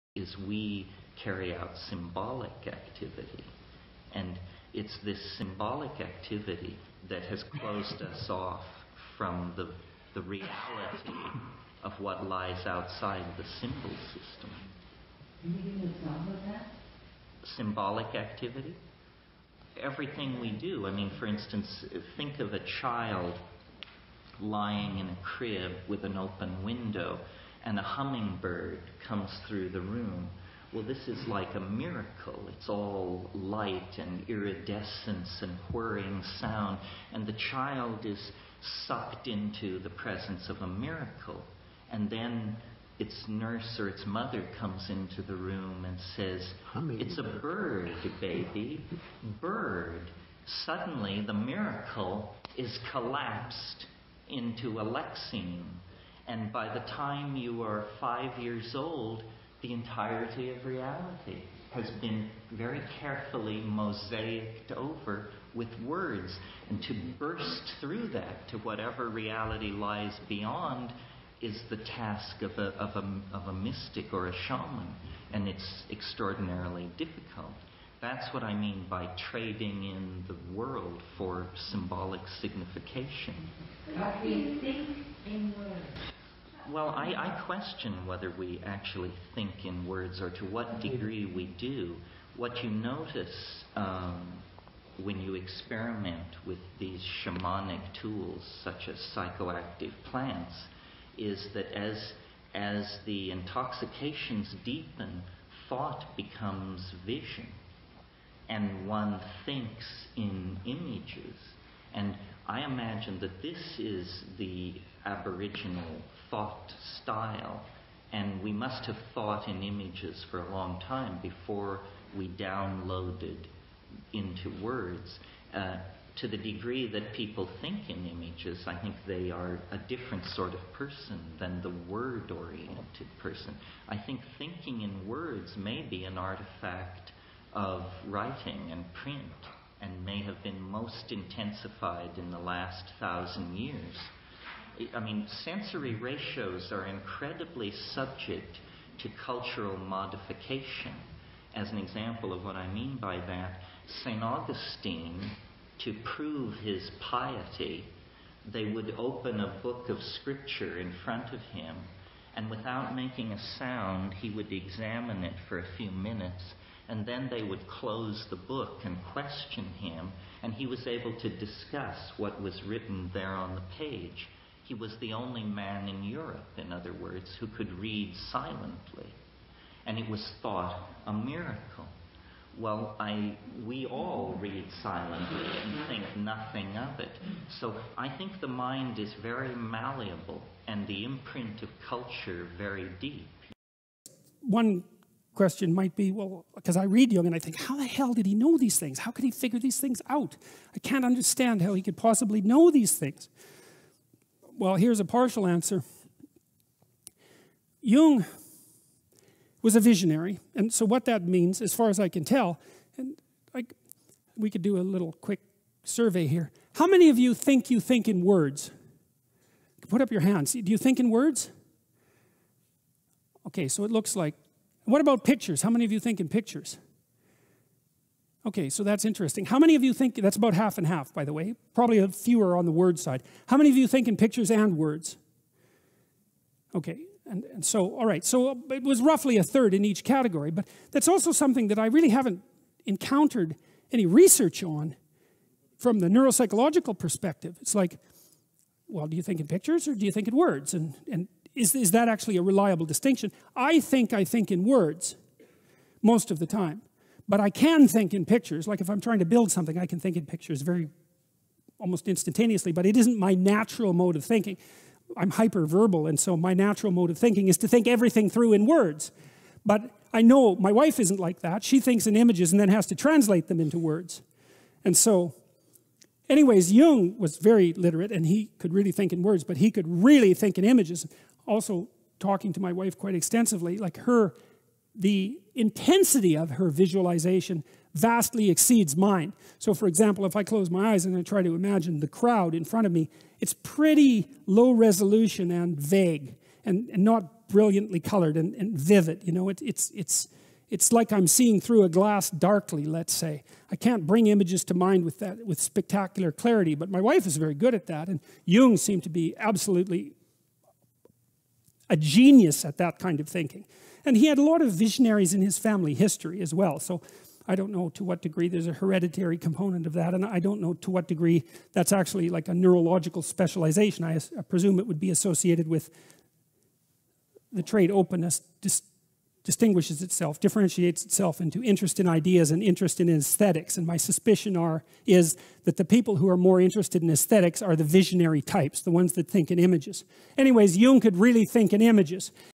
Jordan Peterson Terence McKenna on thinking in words or pictures(GMTA3)
Do great minds think alike? Two fascinating thinkers remark on thinking in words or images...

 Voyage à travers l'impossible (The Impossible Voyage) by Georges Méliès (1904 silent film)